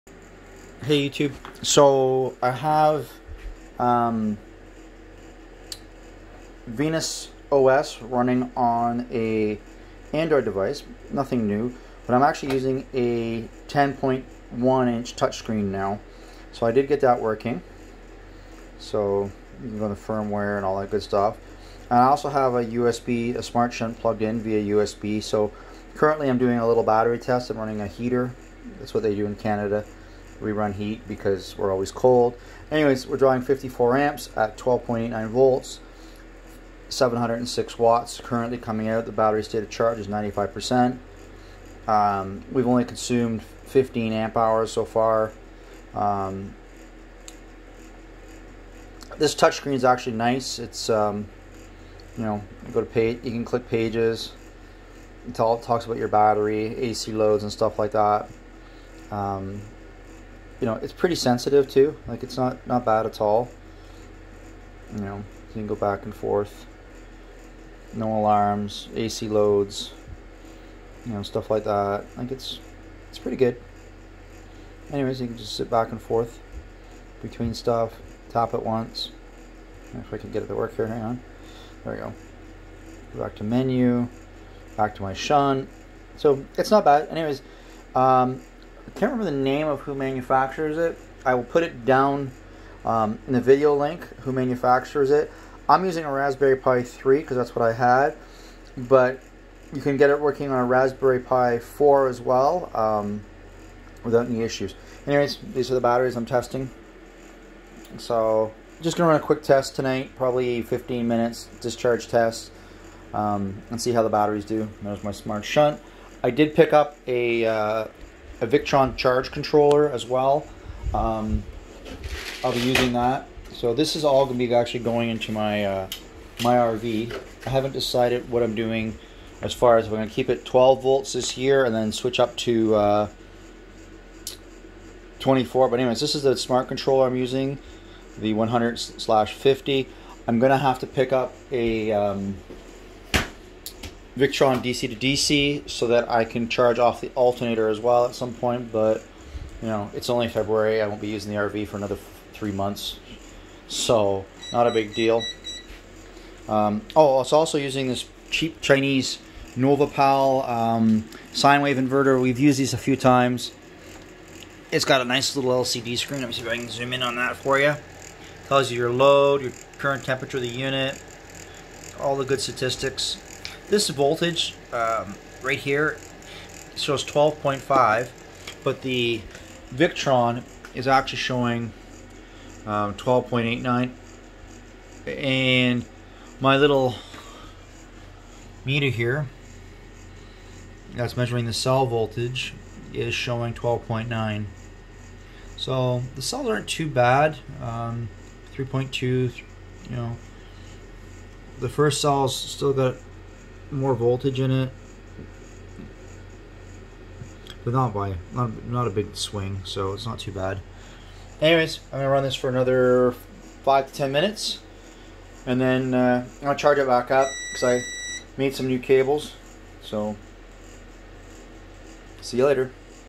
Raspberry Pi - Victron OS with 10.1 Touch Screen with Lifepo4 battery
We have a 10.1 touchscreen running on Victron OS 2.6.   We are also doing a small discharge test of 55 Amps out of a 260AH Lifepo4 battery.

Raspberry Pi 10.1 Inch Touchscreen - SunFounder 10.1" HDMI 1280x800 IPS LCD Touch

Victron SmartShunt

Victron SmartCharge Controller

Novopal 2000w 12V Pure Sine Wave Power Inverter